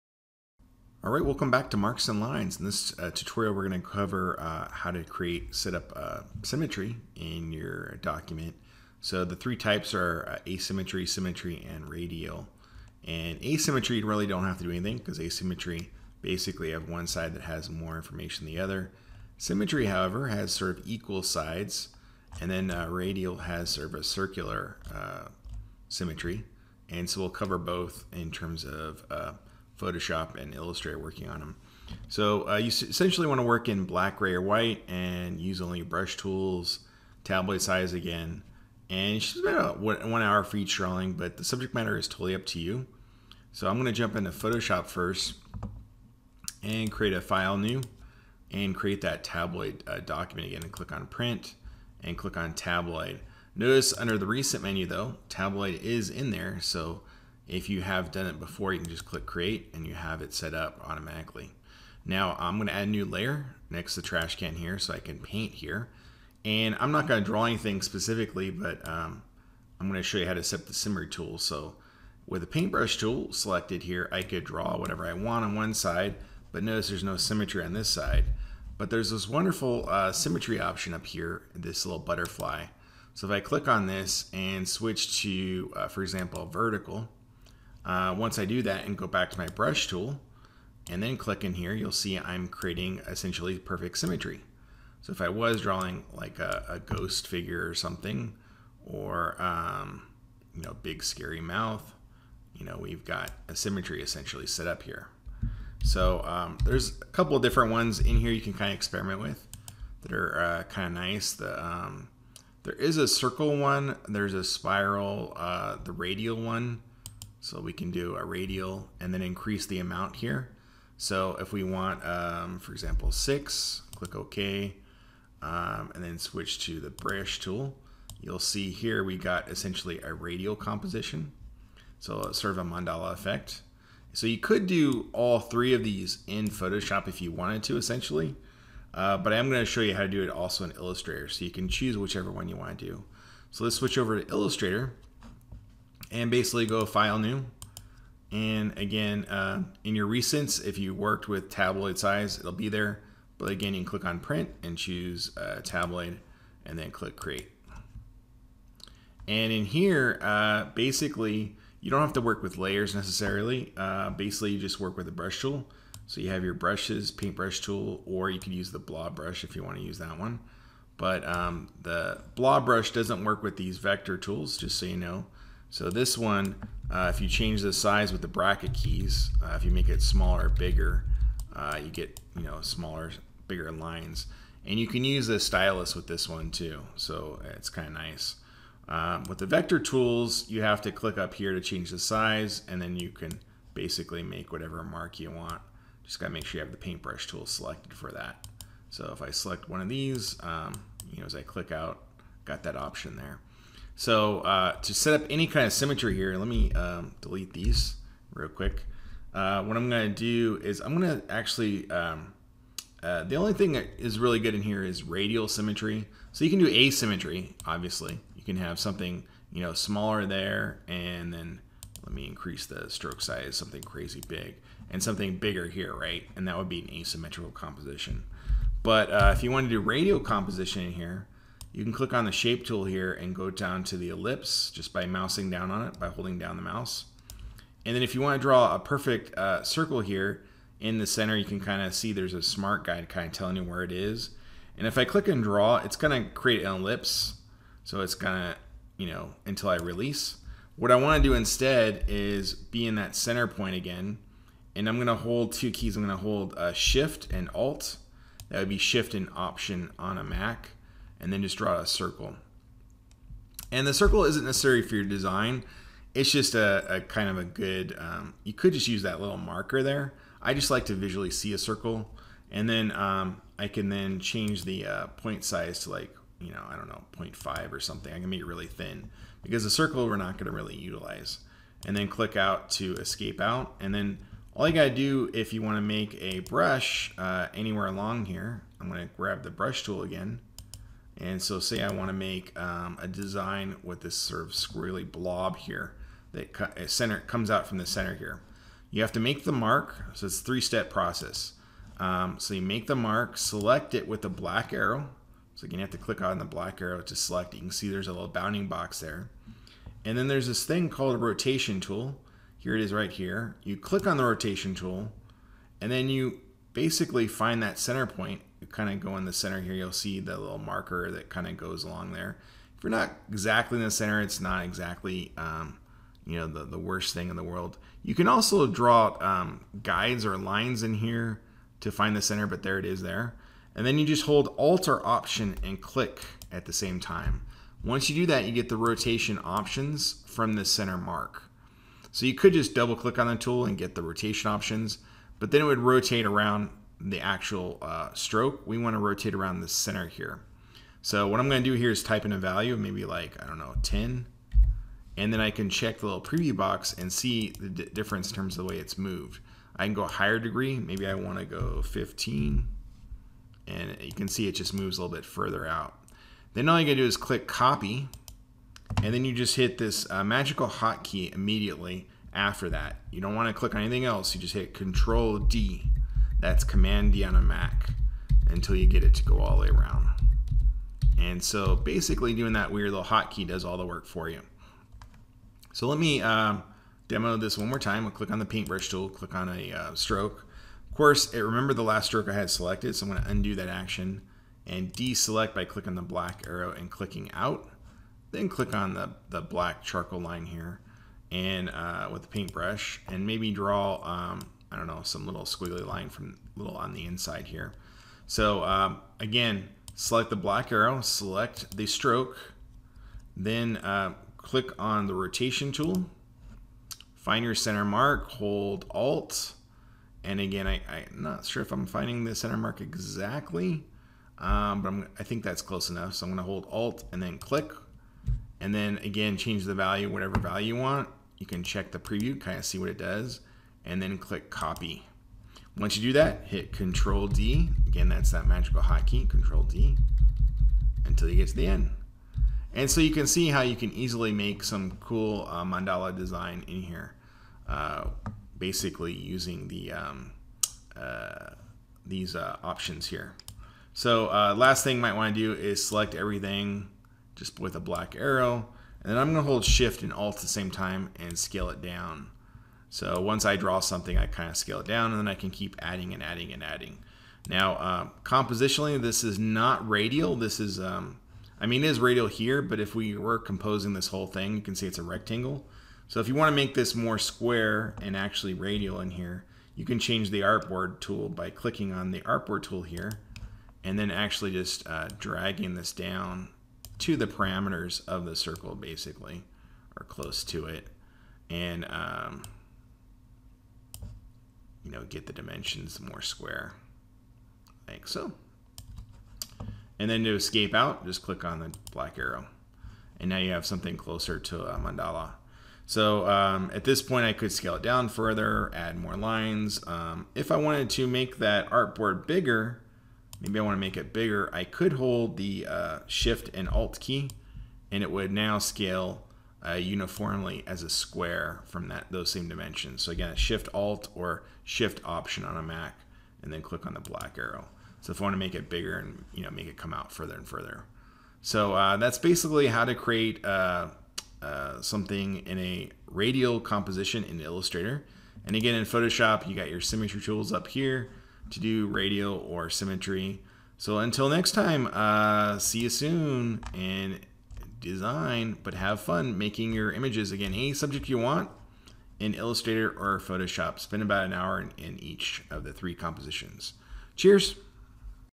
Marks and Lines (Symmetry, Asymmetry, and Radial Balance)
Learn how to use the symmetry tools in Adobe Photoshop and how to use the Rotation Tool to create a Radial composition in Adobe Illustrator!